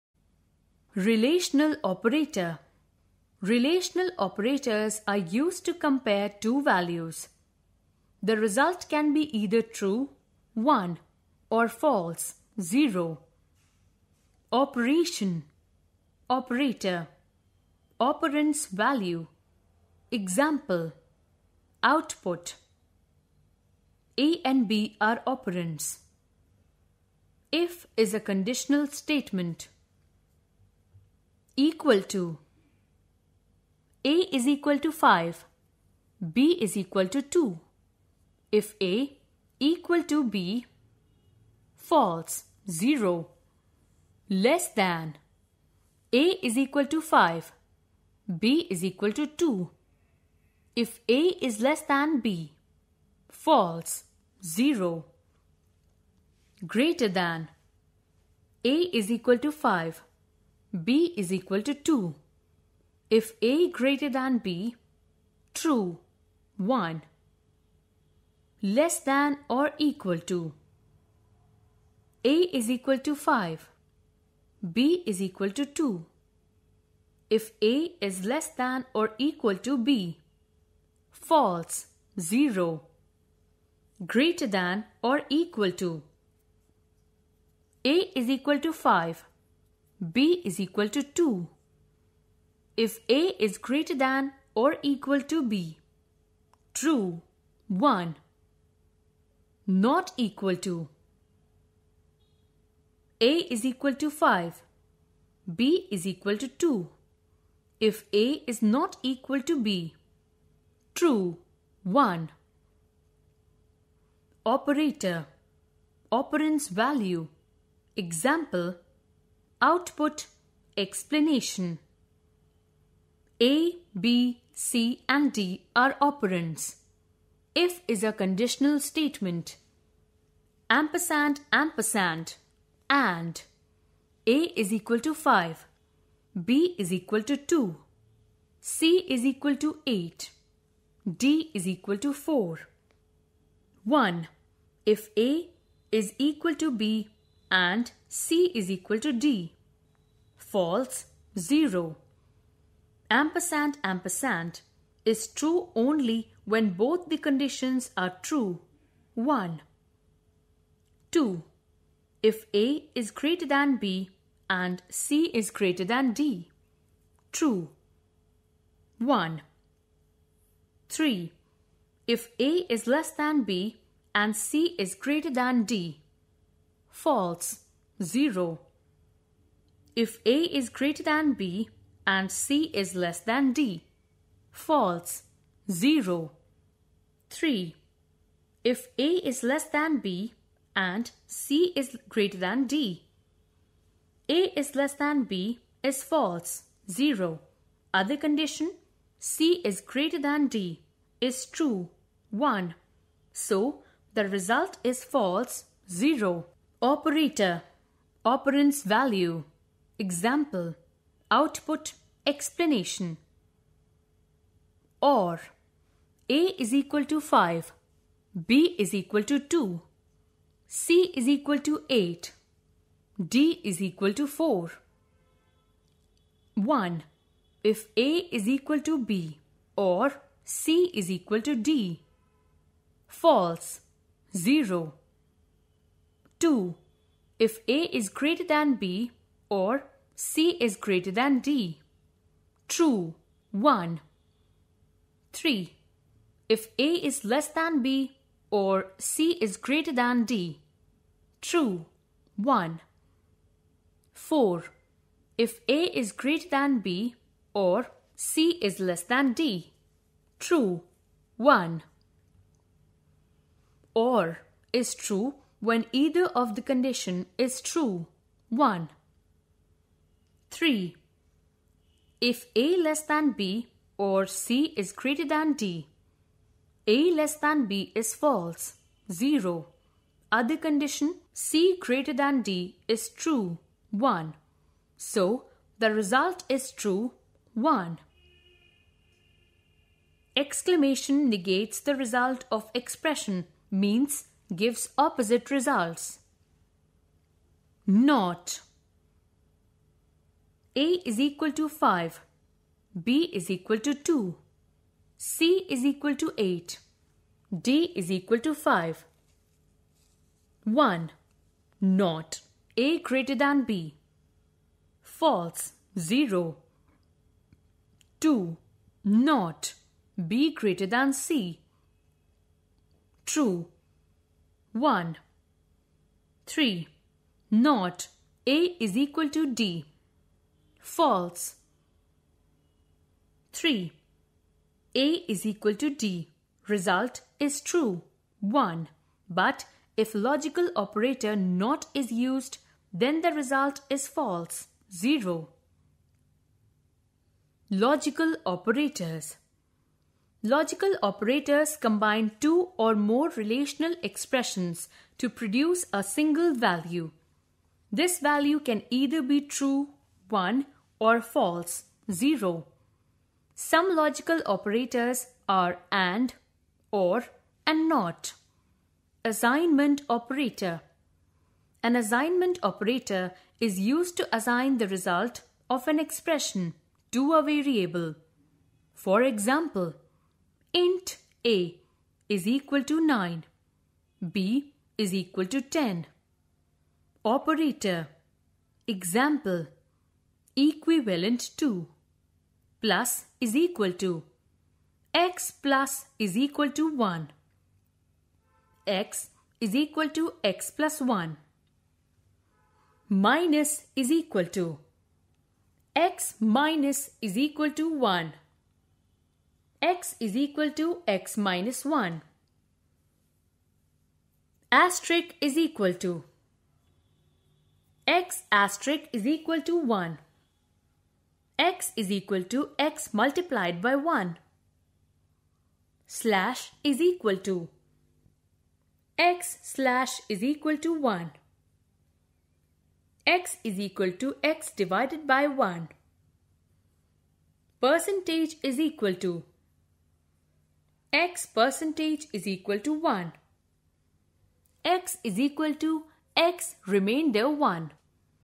Program Coding class-8
This is an animated video from Kriti Educational Videos that explains about operators and types of operators. It shows about relational operators, logical operators and assignment operators. Video uploaded under permission from Kriti Prakashan Pvt Ltd, copyright
The mode of communication is done in English
Contents:
1. Operator
2. Types of Operators
This video is suitable for class 8.